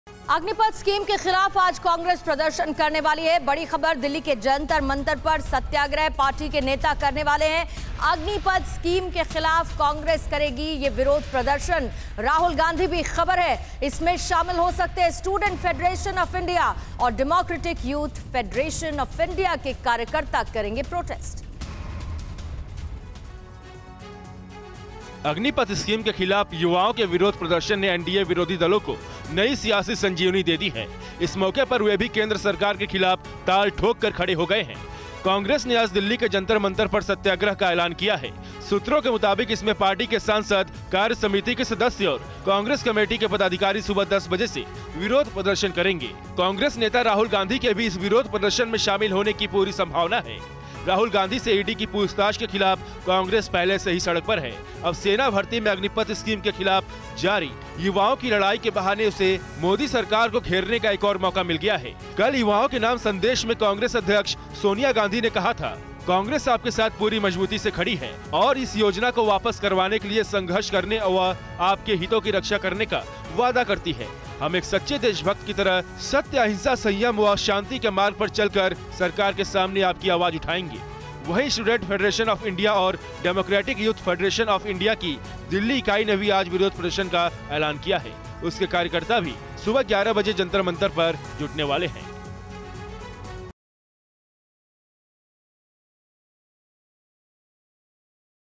Agneepath Scheme के खिलाफ Congress का प्रदर्शन, Dellhi के जंतर-मंतर पर सत्याग्रह करेंगे पार्टी नेता
अग्निपथ स्कीम के खिलाफ युवाओं के विरोध प्रदर्शन ने एनडीए विरोधी दलों को नई सियासी संजीवनी दे दी है। इस मौके पर वे भी केन्द्र सरकार के खिलाफ ताल ठोंककर खड़े हो गए है। कांग्रेस ने आज दिल्ली के जंतर–मंतर पर 'सत्याग्रह' का ऐलान किया है। कांग्रेस नेता राहुल गांधी के भी इस विरोध प्रदर्शन में शामिल होने की पूरी संभावना है.